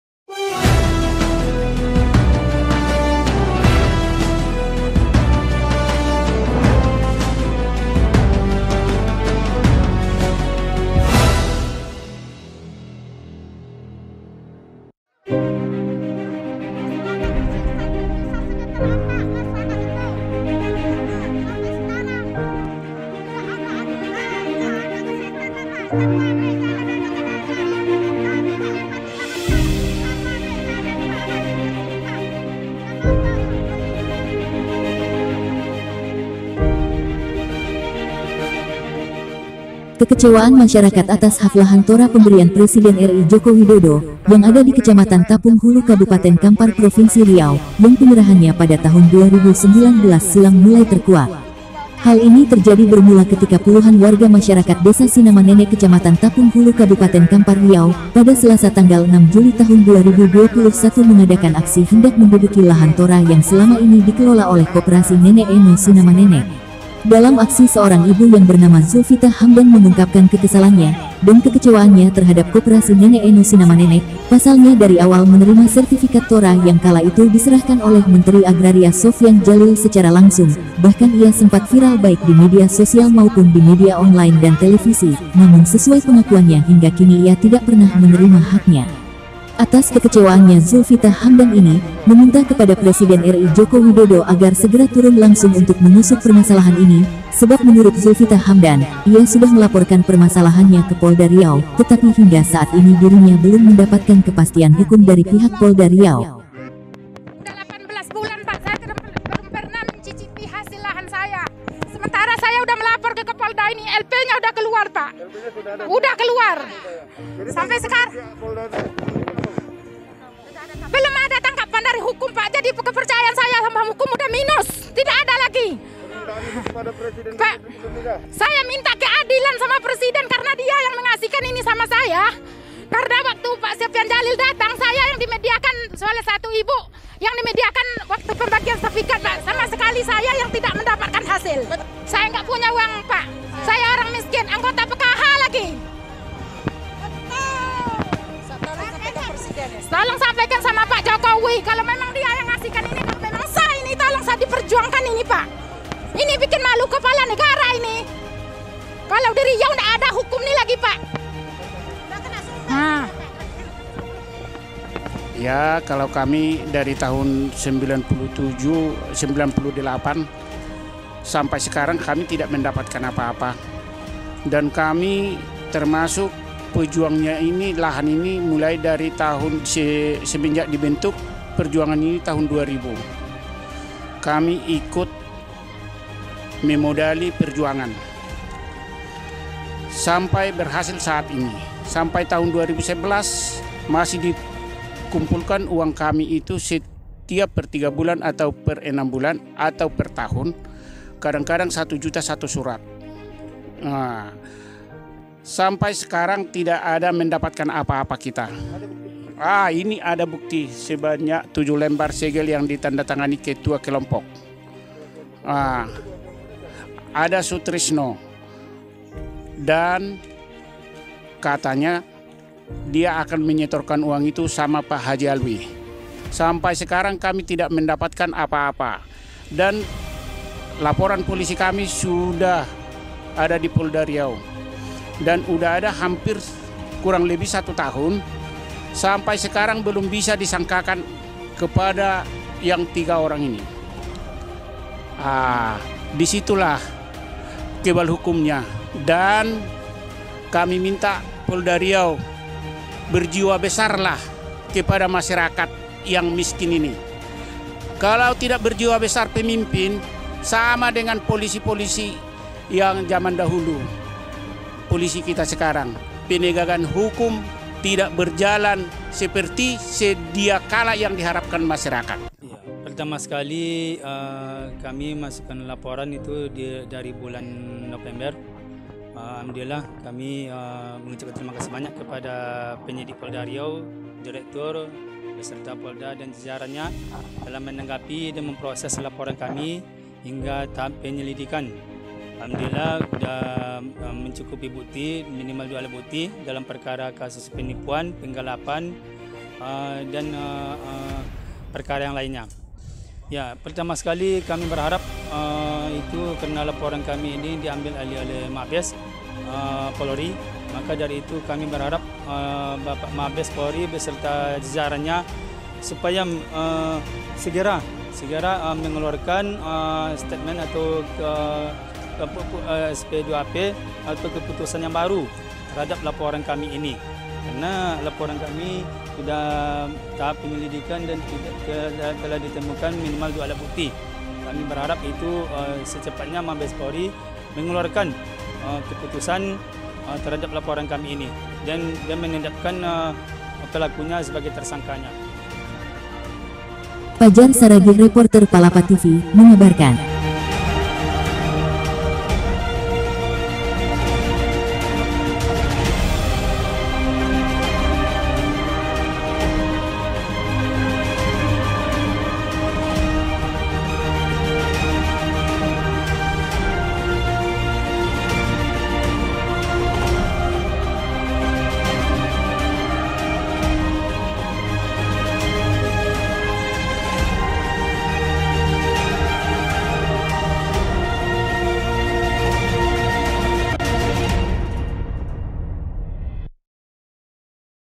PRESIDEN JOKOWI DIMINTA WARGA SINAMA NENEK UNTUK ATASI MASALAH LAHAN TORA DI TAPUNG HULU
Palapa TV,,Kekecewaan Masyarakat atas hak Lahan Tora pemberian Presiden RI Joko Widodo yang ada di kecamatan Tapung Hulu kabupaten Kampar Provinsi Riau, yang penyerahannya pada tahun 2019 silam mulai terkuak
Hal ini terjadi bermula ketika puluhan warga masyarakat Desa Sinama Nenek kecamatan Tapung Hulu kabupaten Kampar, Riau pada Selasa 6 Juli 2021 mengadakan aksi hendak menduduki lahan tora yang selama ini dikelola oleh Koperasi Nenek Eno Sinamanenek.
Dalam aksi seorang ibu yang bernama Zulvita Hamdan mengungkapkan kekesalannya dan kekecewaannya terhadap Koperasi Nenek Eno Sinamanenek,pasalnya dari menerima Sertifikat Tora yang kala itu diserahkan oleh Menteri Agraria Sofyan Djalil secara langsung,bahkan ia sempat viral,baik di media sosial maupun di media online dan televisi,namun sesuai pengakuannya bahwa ia tidak pernah menerima haknya.